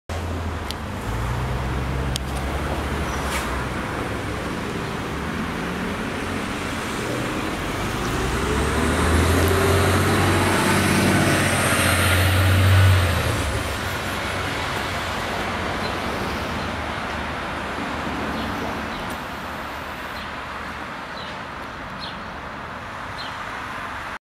NJT MCI D4500CT CNG #7227 on the 128 to New York (Outside) in HD
New York City bound NJT MCI D4500CT CNG Coach on Boulevard East & Bonn Place in Weehawken heading inbound to the Big Apple on a cloudy and cool Thursday Morning.  Nice, smooth takeoff on this CNG Coach!!!  Available in HD.

Recorded with my Canon Powershot SX170is

Powertrain of NJT MCI D4500CT CNG #7227:
Engine: Cummins Westport ISL-G
Transmission: Allison B500R Gen IV
I JUST LOVE THESE NEW NJT MCI D4500CT CNG Commuter Coaches!!!
Eco-Friendly